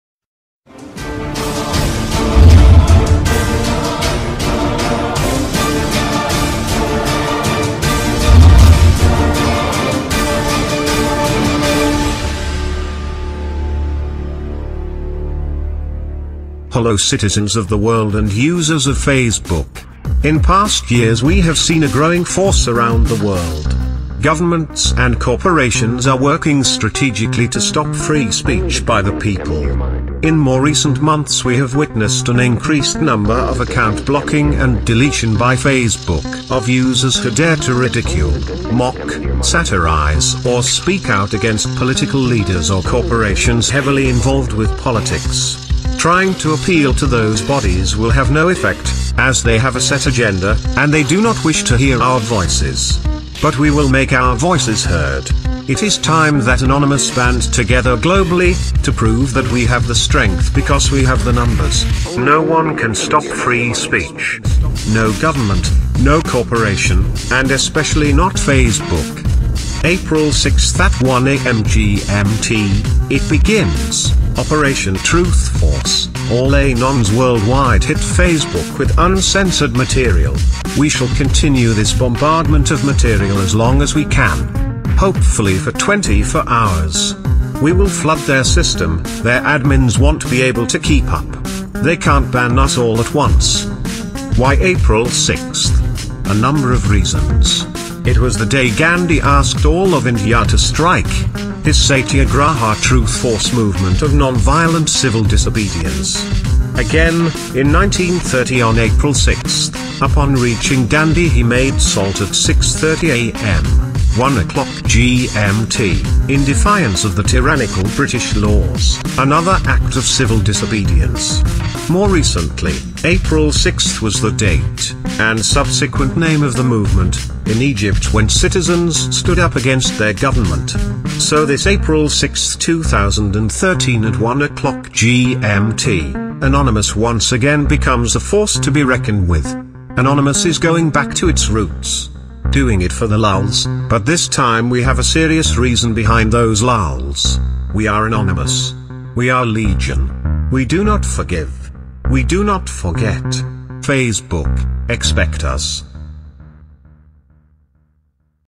Anonymous: Operation Truth Force - April, 6 2013
TRANSCRIPT
Anonymous - OpTruthForce - April 6 2013

Hello citizens of the World and users of Facebook.

In past years we have seen a growing force around the world. Governments and corporations are working strategically to stop Free Speech by the people.

In more recent months we have witnessed an increased number of account blocking and deletion by Facebook, of users who dare to ridicule, mock, satirize or speak out against political leaders or corporations heavily involved with politics. Trying to appeal to those bodies will have no effect, as they have a set agenda and they do not wish to hear our voices. But we will make our voices heard.

It is time that Anonymous band together globally to prove that we have the strength, because we have the numbers. NO ONE CAN STOP FREE SPEECH. No government, no corporation, and especially not Facebook.

APRIL 6 at 1 AM GMT it begins: OpTruthForce.

All Anons worldwide hit Facebook with uncensored material. We shall continue this bombardment of material as long as we can - hopefully 24 hours. We will flood their system, their admins wont be able to keep up. THEY CAN'T BAN US ALL AT ONCE!

Why April 6th ? A number of reasons: It was the day Ghandi asked all of India to strike, his Satyagraha "TRUTH FORCE" movement of NON VIOLENT CIVIL DISOBEDIENCE. Again, in 1930 on April 6, upon reaching Dandi he made Salt at 6:30 AM (1:00 GMT) in defiance of the tyrannical British laws, another act of civil disobedience. More recently, April 6 was the date, and subsequent name of the movement, in Egypt, when citizens stood up against their government.

So this April 6 2013 at 1:00 GMT, Anonymous once again becomes a force to be reckoned with. Anonymous is going back to its roots. Doing it for the lulz. But this time we have a serious reason behind those lulz.

Facebook, Expect us.